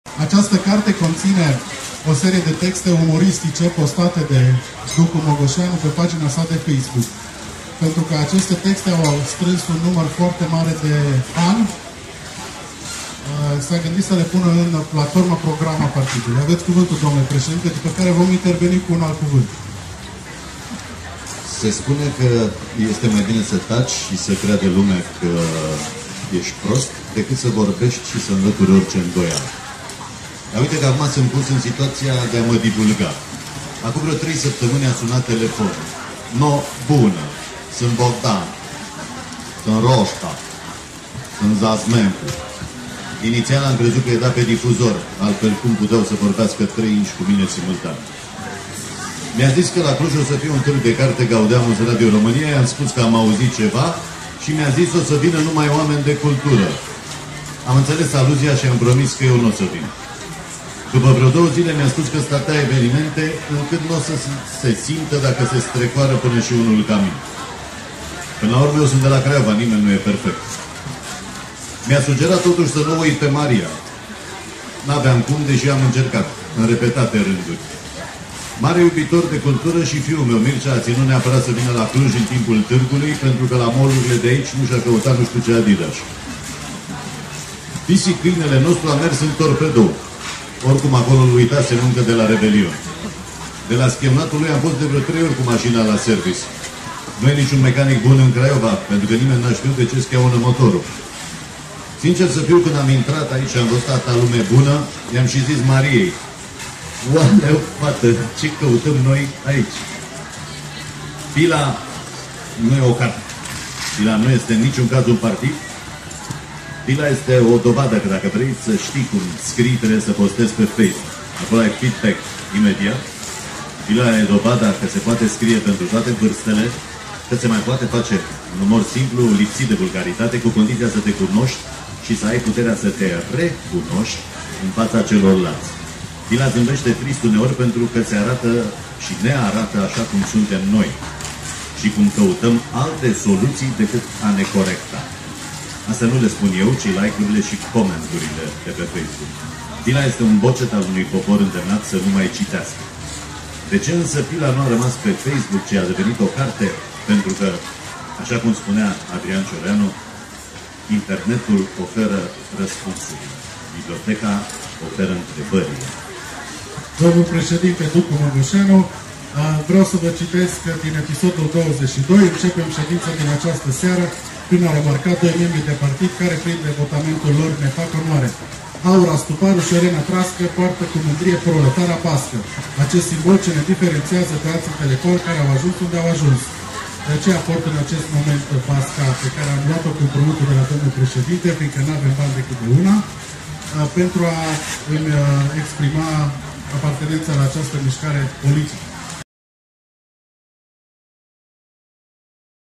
Ducu Mogoseanu- PILA-lansare Gaudeamus Cluj 2016
Ducu Mogoșeanu, managerul Radio Craiova, a lansat P.I.L.A, Partidul Intelectualilor Liber Angajați, la Gaudeamus Cluj 2016 alături de Bogdan Roșca, managerul Radio Cluj.